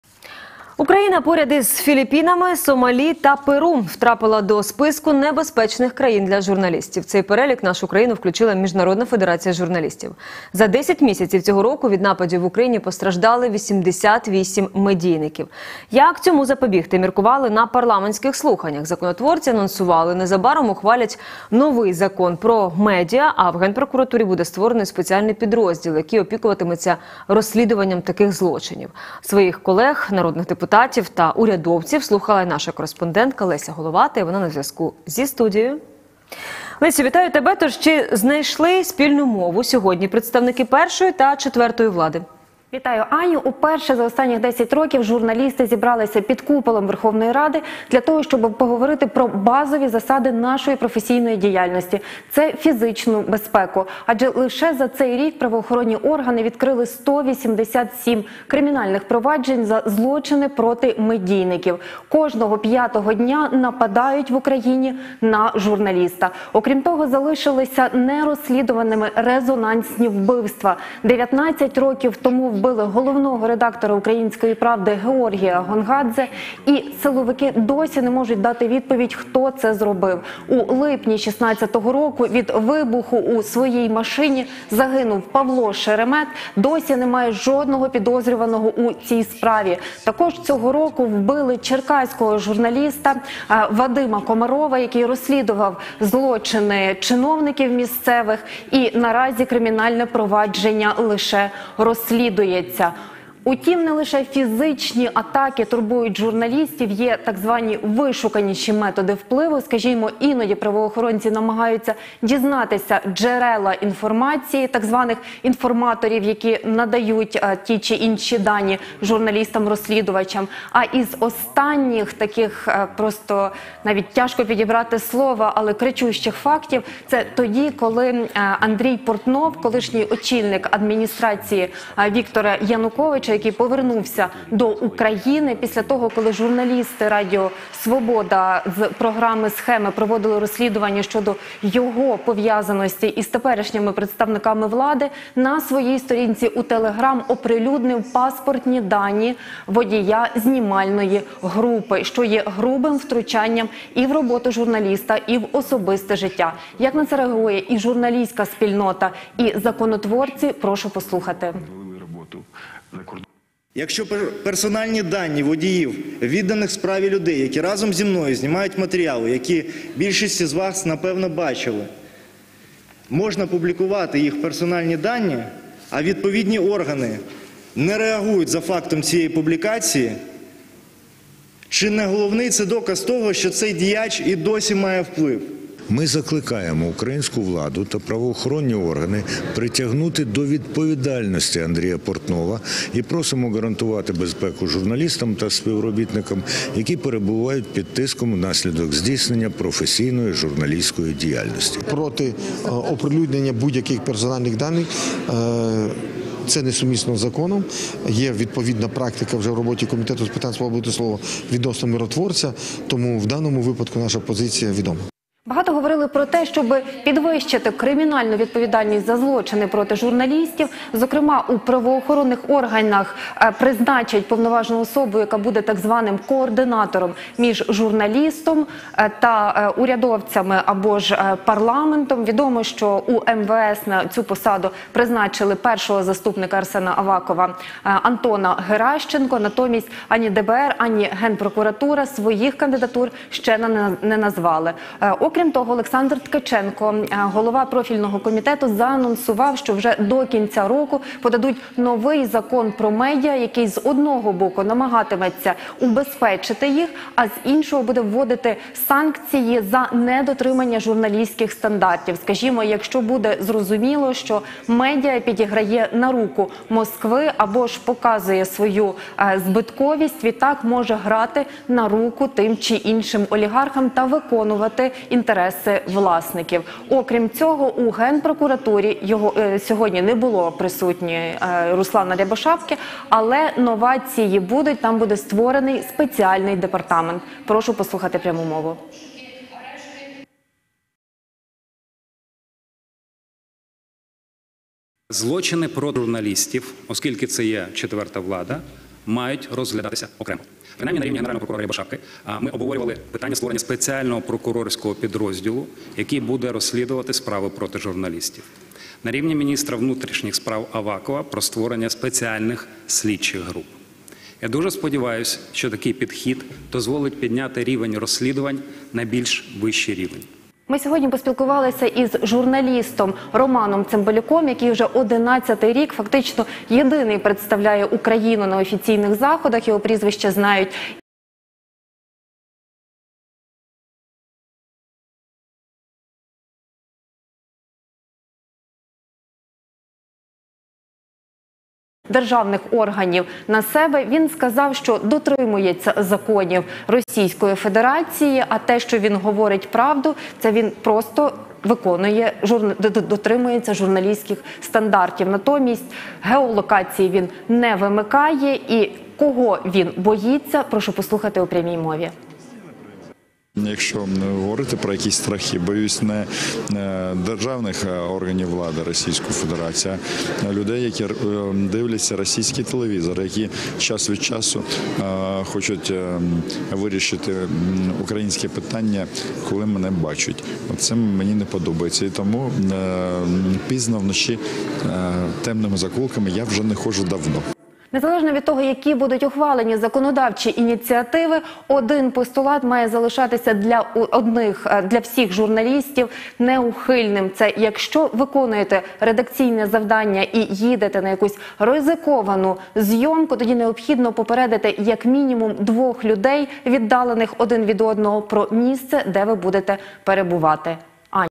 Поряд з Філіппінами, Сомалі та Перу: чим завершилися слухання в Раді з питань безпеки журналістів
Україна поряд із Філіппінами, Сомалі та Перу втрапила до списку небезпечних країн для журналістів. В цей перелік нашу країну включила міжнародна федерація журналістів. За 10 місяців 2019 року від нападів в Україні постраждали 88 медійників. Як цьому запобігти - міркували на парламентських слуханнях. Законотворці анонсували - незабаром ухвалять новий закон про медіа, а в генпрокуратурі буде створений спеціальний підрозділ, який опікуватиметься розслідуванням таких злочинів.